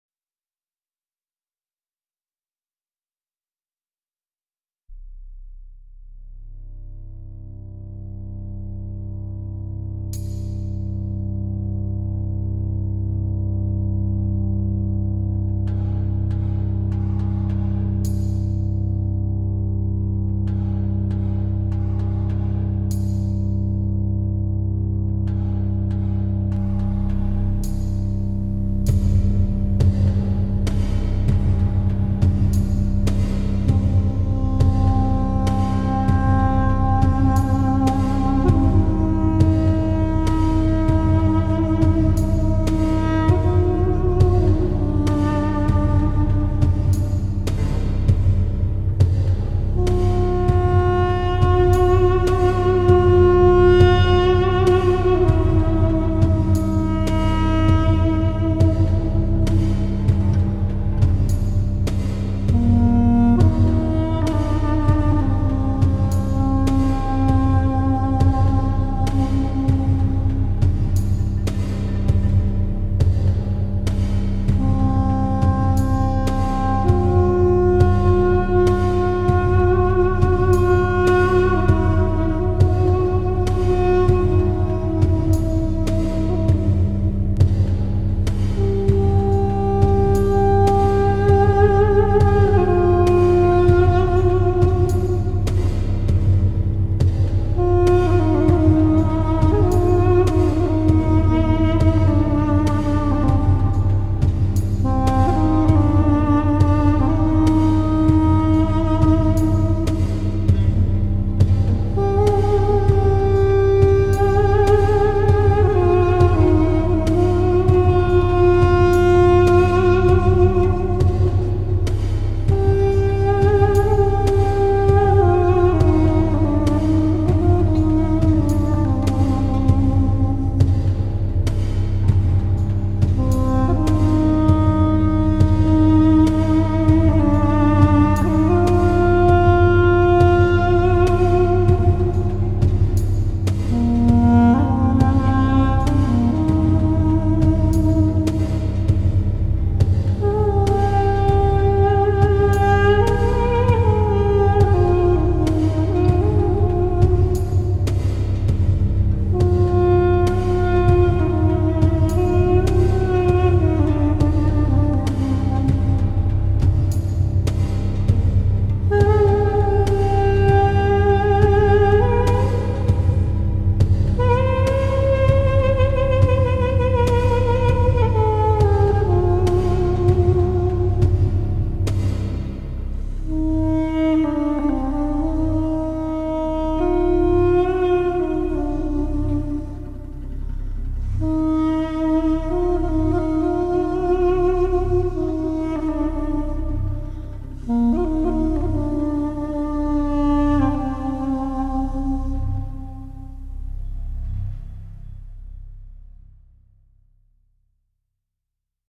Memories - A Duduk improvisation
A short piece I wrote and recorded for the Duduk a while back.